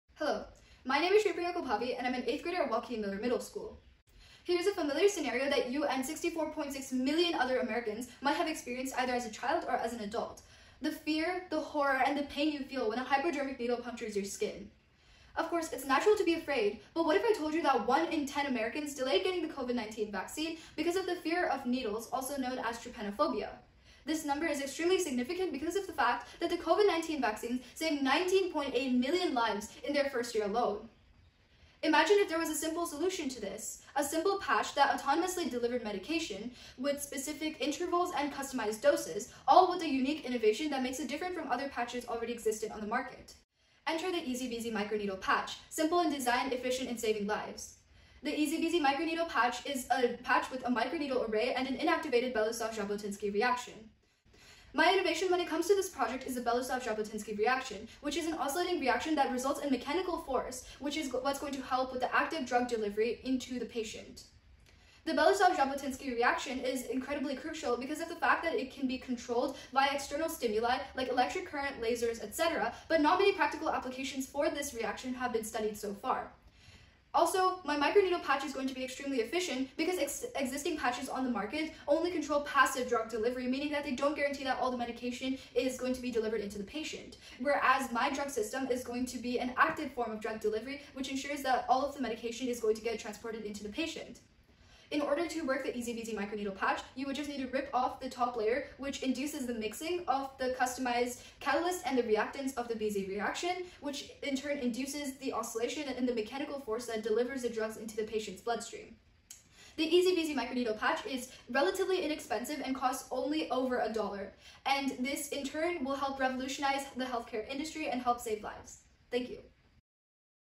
3MYSC Submission - Shripriya Kalbhavi
Meet Shripriya. She developed EasyBZ, a cost-effective microneedle patch that allows for self-automated drug delivery without pills or needles.

To learn more about the 2023 3M Young Scientist Challenge Top 10 Finalists, visit youngscientistlab.com.